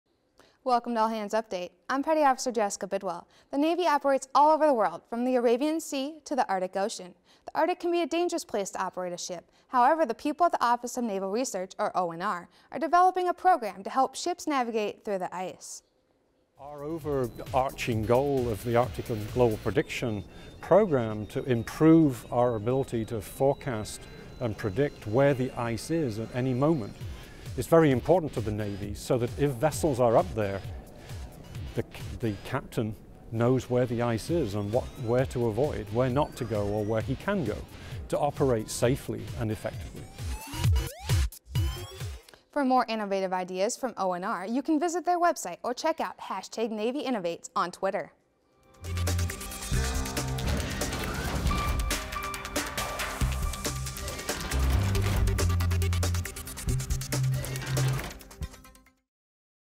ONR Develops Program to Assist Ships While Navigating Through the Ice in the Arctic
Office of Naval Research is developing a program to assist ships navigating through the ice in the Arctic.